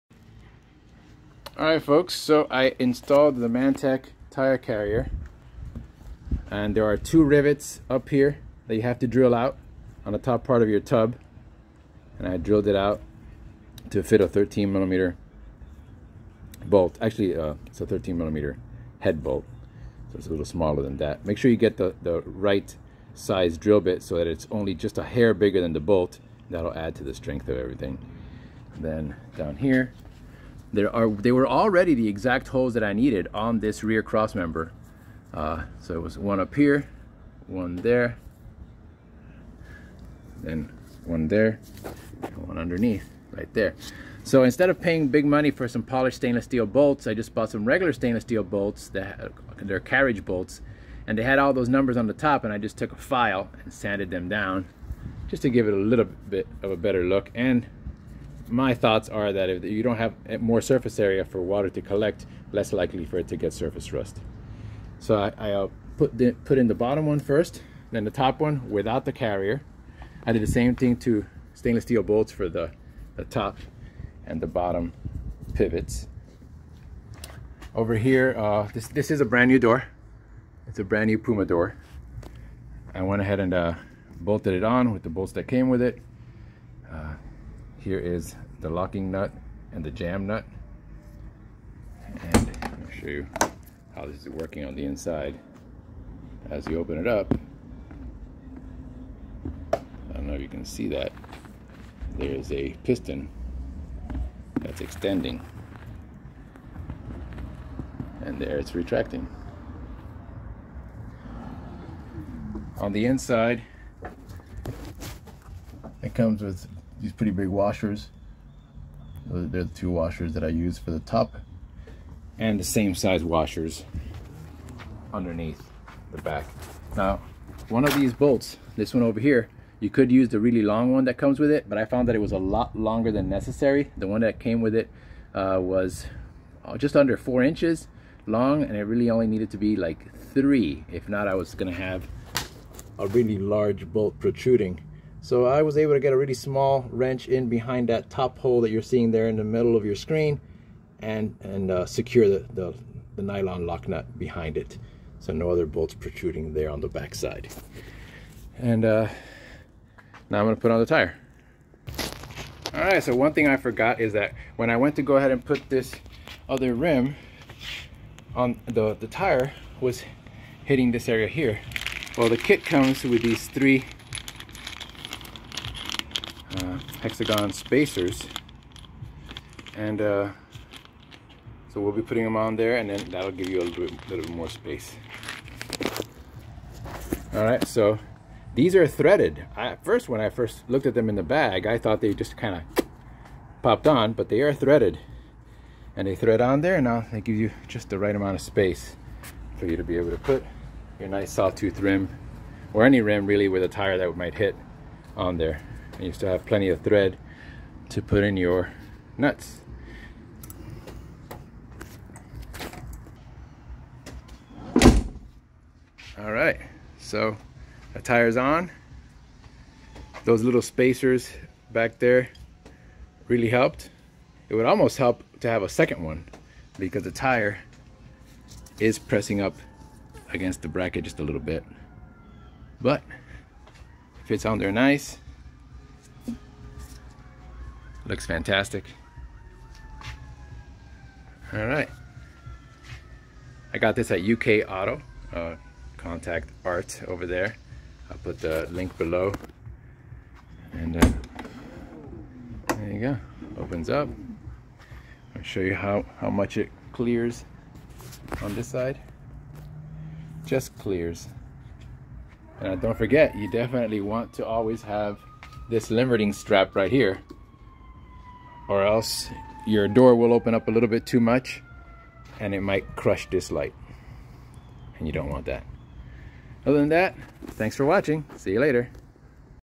Land Rover Defender Mantec Tire Carrier
I just installed the Mantec Land Rover rear tire carrier on my Defender 90.
Sorry I don't have a more detailed step by step video but hopefully this helps. May times all we DIY'ers need is to look at something several times to get an understanding of how to do something. You tube is always a great asset and teaching tool.